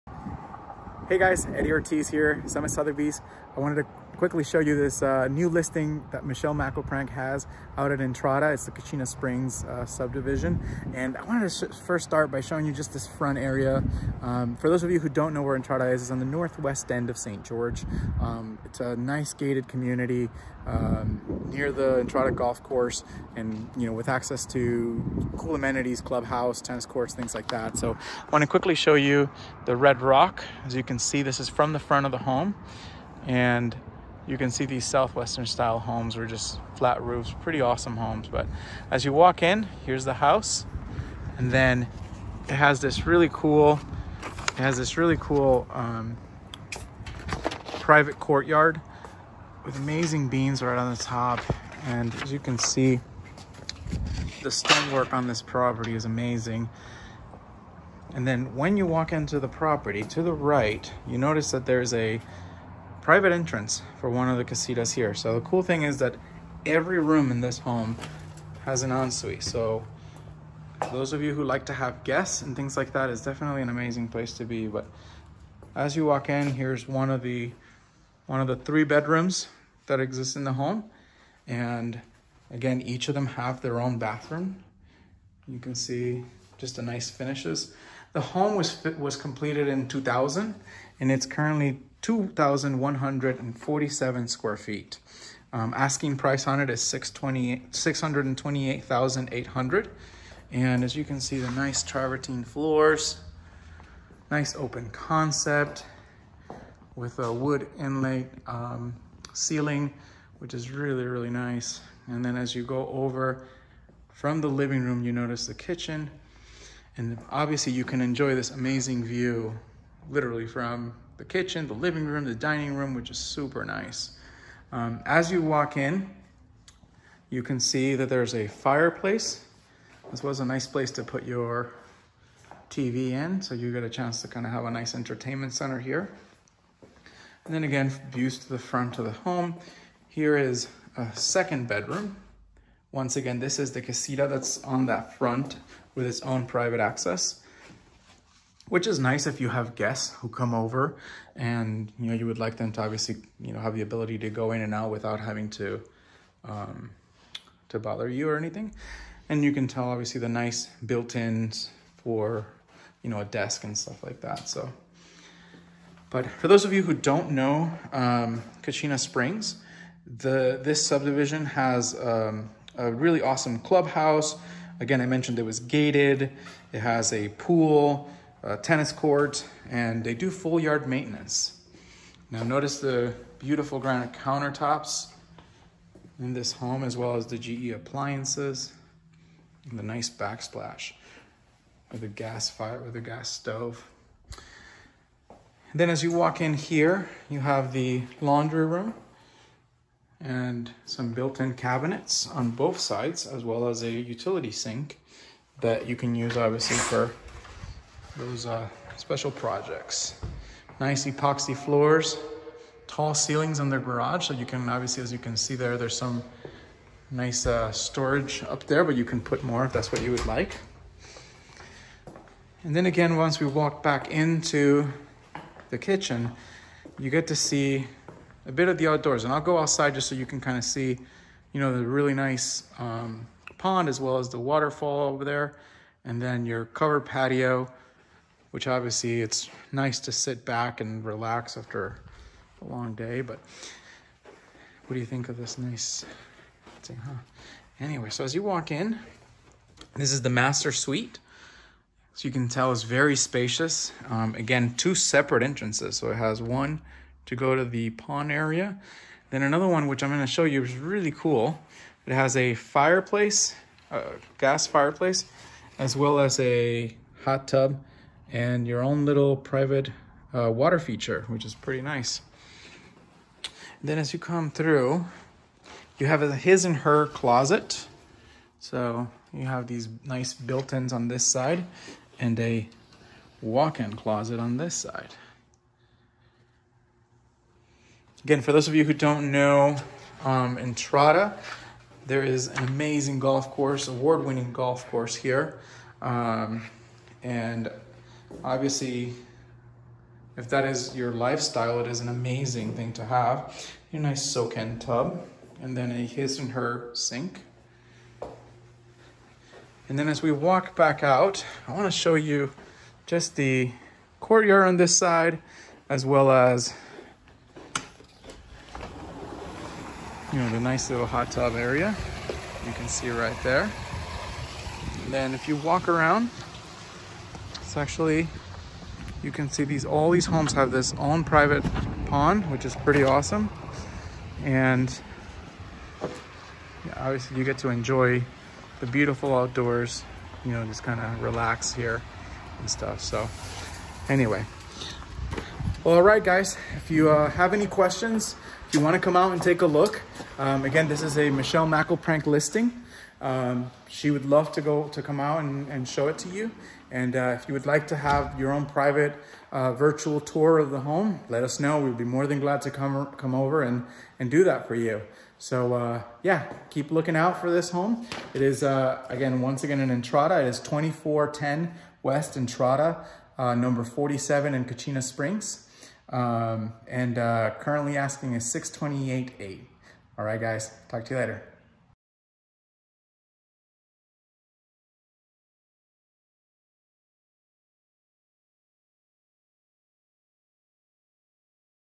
Virtual Tour | Own Your Own Retreat In Entrada
Kachina Springs at Entrada
2410 W Entrada Trail #47 | St. George, Utah
3 Bedrooms | 3 Bathrooms | 2 Car Garage | 2,147 SqFt

Coveted location on the pond in the private gated community of Kachina Springs. This 3 bedroom, 3 bathroom, 2,147 sq. ft. home, features a private patio with a built-in outdoor Kiva fireplace and relaxing spa right off the master bedroom. Located near all the Entrada amenities, path to the fitness center, clubhouse, pools and walking trails. From the wood beams of the inviting courtyard, to an open floor plan with floor to ceiling windows, this beautiful home showcases a cascading waterfall and backyard pond. The spacious master suite has two closets, large soaking tub and a patio access to a Kiva fireplace and jacuzzi. Each quest room has its own ensuite bathroom and private entrance. All boasting a magnificent red mountain view. You will want to see this home!

Listing Agent
Michelle Mackelprang

Virtual Tour By
Eddy Ortiz, PSA, SRS

Listing Brokerage
Summit Sotheby's International Realty